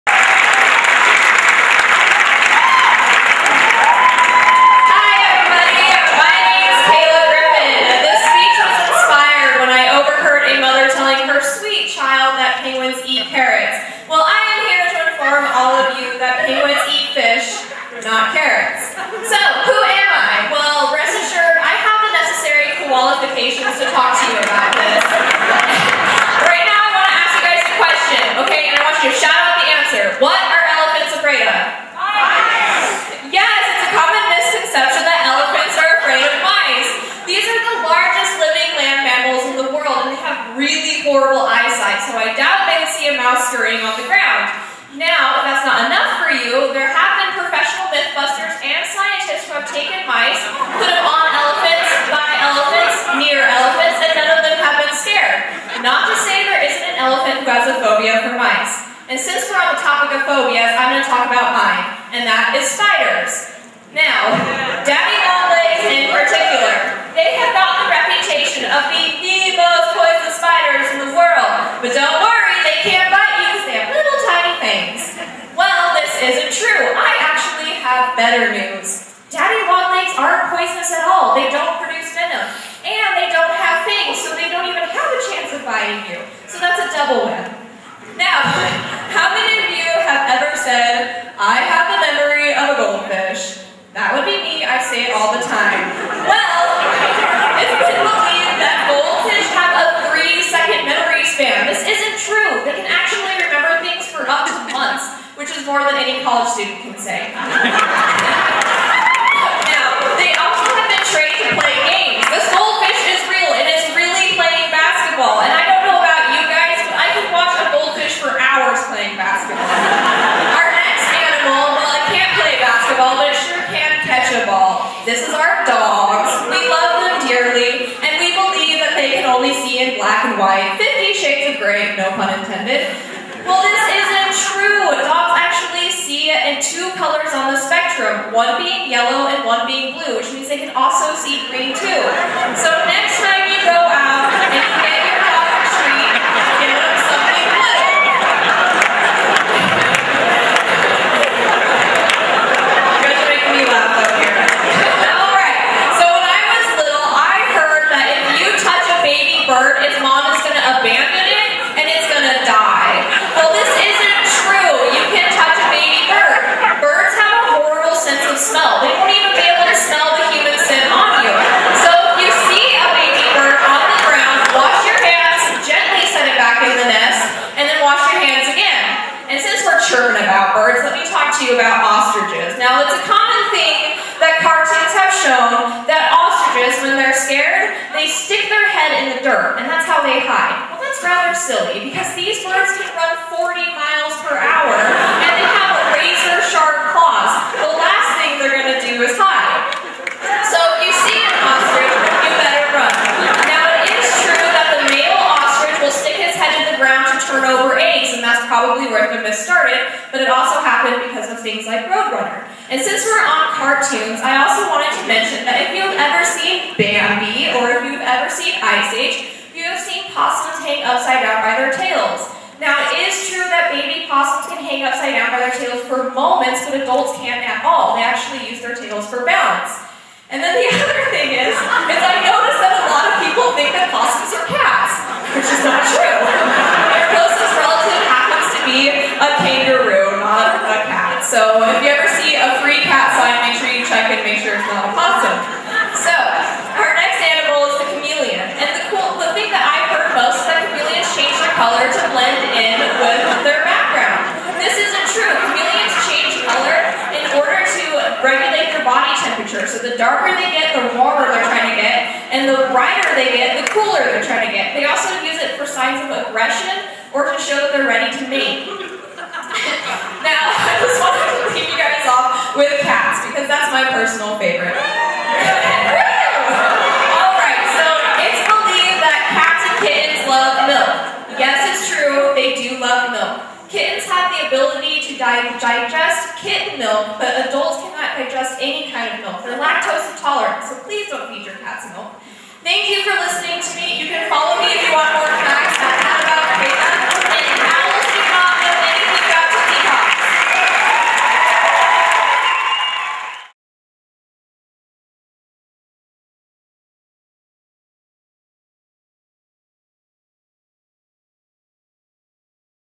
Penguins eat fish, not carrots!
by Kayla Griffin

I have heard so many crazy things that parents teach their children about animals that are 100% false! I want to spend my 5 minutes debunking as many of those fake facts as possible! (example: I over heard this woman telling her son that penguins eat lettuce and carrots...well I hate to break it to everybody, but penguins actually eat fish).